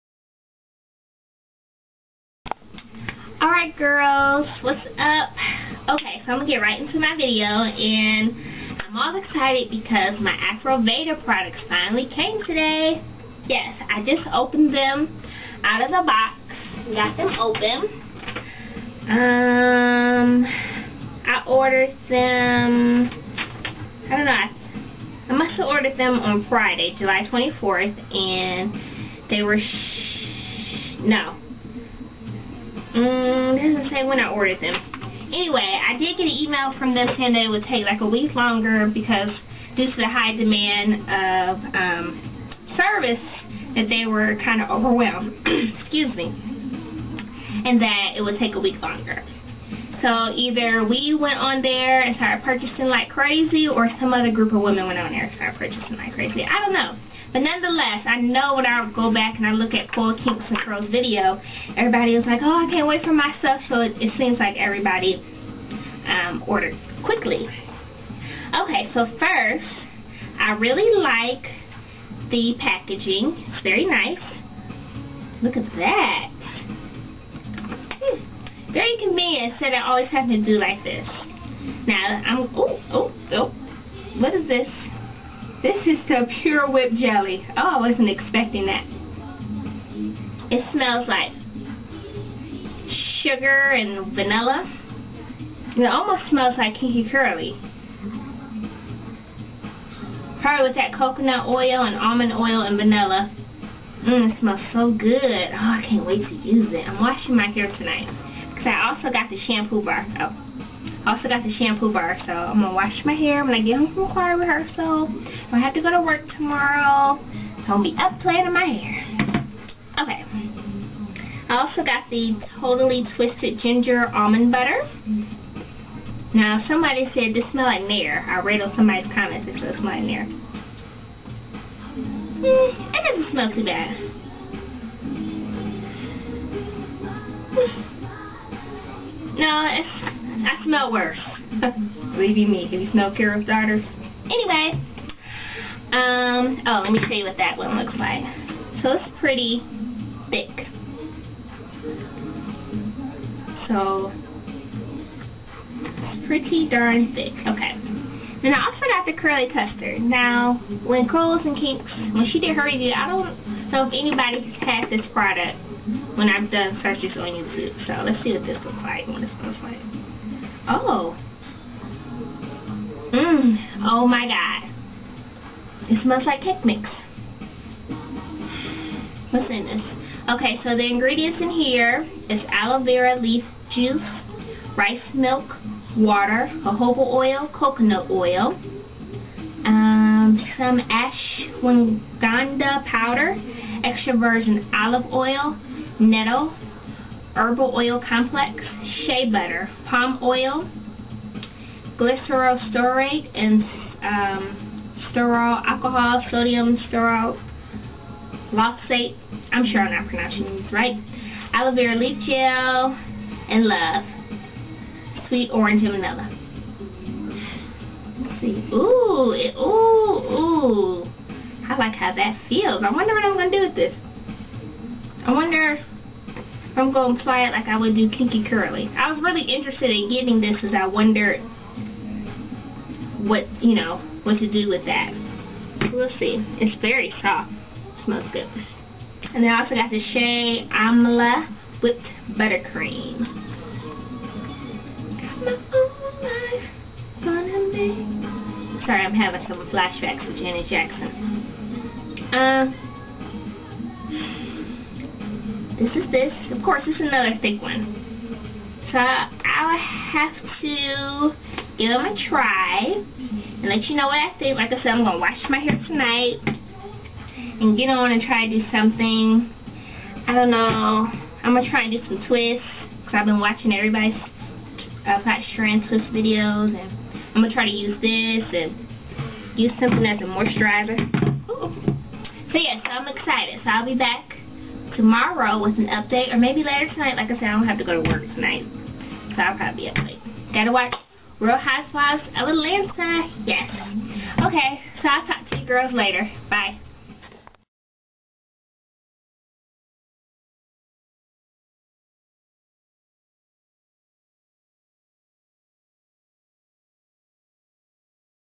Afroveda Hair Haul
Just talking about what products I ordered from afroveda.com . I got the Pur Whipped Jelly, Totally Twisted, the Curly Custard, and Amla Butter.